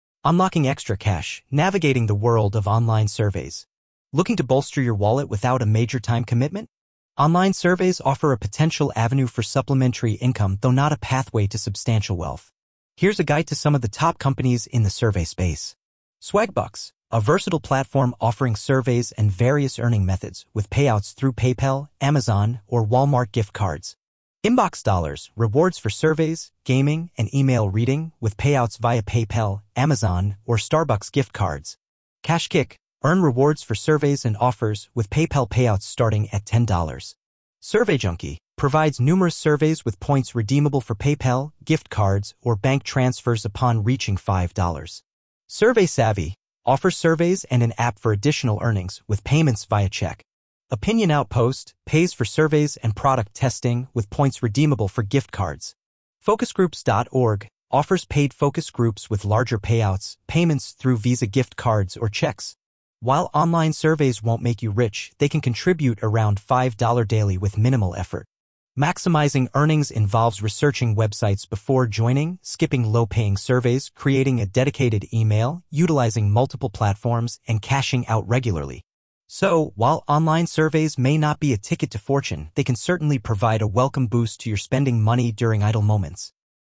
how to make money taking surveys
how to make money taking surveys "Unlocking Extra Cash: Navigating the World of Online Surveys"
Looking to bolster your wallet without a major time commitment? Online surveys offer a potential avenue for supplementary income, though not a pathway to substantial wealth. Here's a guide to some of the top companies in the survey space:

Swagbucks: A versatile platform offering surveys and various earning methods, with payouts through PayPal, Amazon, or Walmart gift cards.

InboxDollars: Rewards for surveys, gaming, and email reading, with payouts via PayPal, Amazon, or Starbucks gift cards.

KashKick: Earn rewards for surveys and offers, with PayPal payouts starting at $10.

SurveyJunkie: Provides numerous surveys with points redeemable for PayPal, gift cards, or bank transfers upon reaching $5.

SurveySavvy: Offers surveys and an app for additional earnings, with payments via check.

OpinionOutpost: Pays for surveys and product testing, with points redeemable for gift cards.

FocusGroups.org: Offers paid focus groups with larger payouts, payments through Visa gift cards or checks.

While online surveys won't make you rich, they can contribute around $5 daily with minimal effort. Maximizing earnings involves researching websites before joining, skipping low-paying surveys, creating a dedicated email, utilizing multiple platforms, and cashing out regularly.

So, while online surveys may not be a ticket to fortune, they can certainly provide a welcome boost to your spending money during idle moments.